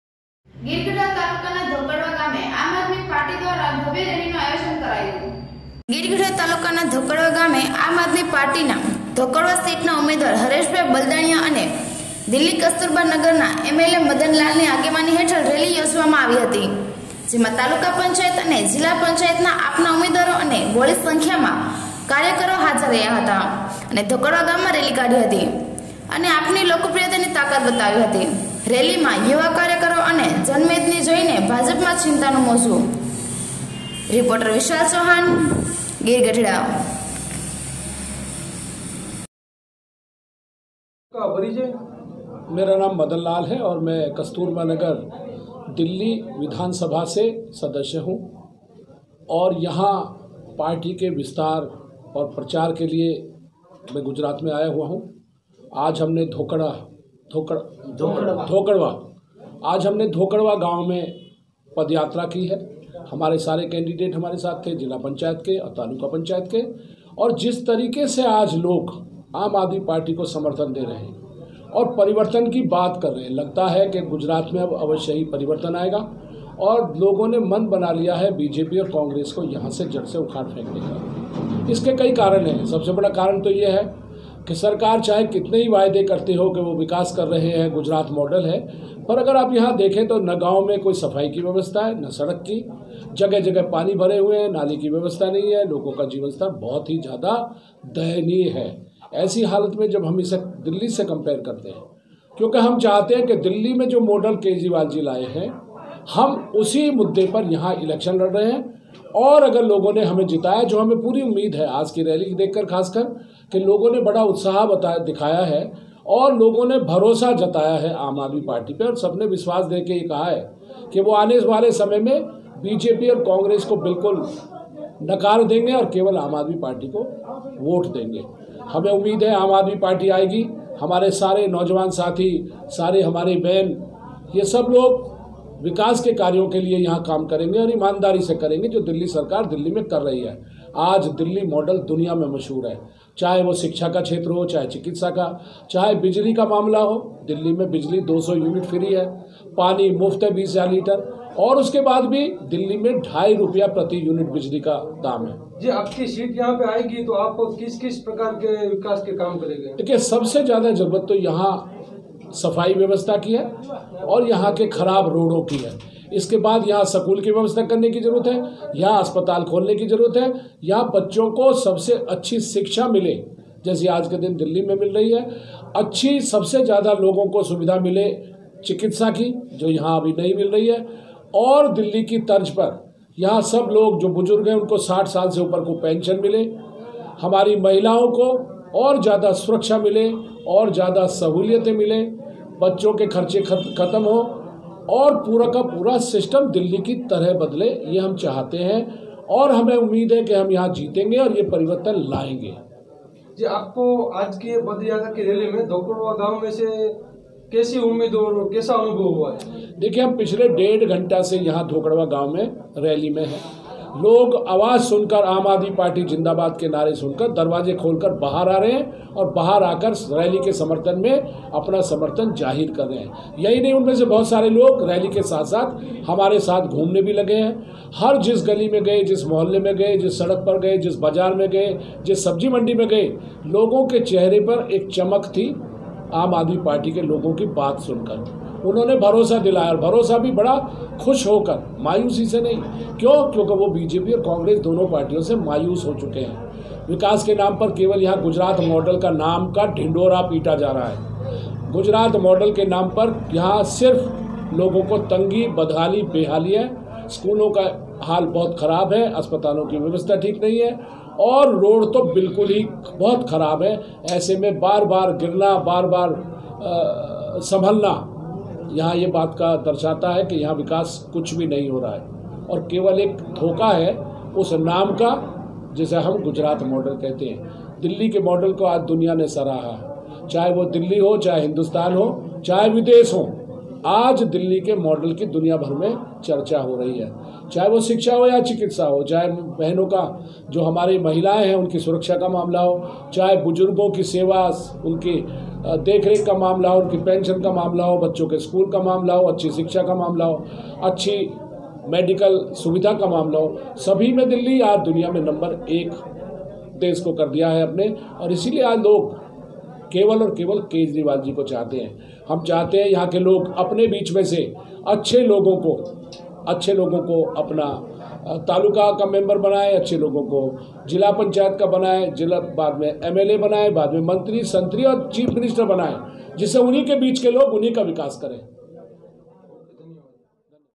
GIR-SOMNATH ગીર ગઢડા તાલુકાના ધોકડવા ગામે આમ આદમી પાર્ટી દ્વારા ભવ્ય રેલીનું આયોજન કરાયું હતું
ગીર ગઢડા તાલુકાના ધોકડવા ગામે આમ આદમી પાર્ટી દ્વારા ભવ્ય રેલીનું આયોજન કરાયું હતું

ગીર ગઢડા તાલુકાના ધોકડવા ગામે આમ આદમી પાર્ટીના ધોકડવા સીટ નાં ઉમેદવાર હરેશ ભાઈ બલ દાનીયા અને દિલ્હી કસ્તુરબા નગર નાં  એમ એ લે મદન લાલ ની આગેવાની હેઠળ રેલી યોજવામાં આવી હતી
જેમાં તાલુકા પંચાત અને જિલ્લા પંચાયત નાં આપ ના ઉમેદવારો અને બહોળી સંખ્યામાં કાર્યકરો હાજર રહ્યા હતા અને
ધોકડવા ગામમાં રેલી કાઢી હતી
અને આપ ની લોકપ્રિયતા ની તાકાત બતાવી હતી
રેલીમાં યુવા કાર્યકરો અને જન મેદની જોઈ ભાજપ માં ચિંતા નું મોજું

રિપોર્ટર . વિશાલ ચૌહાણ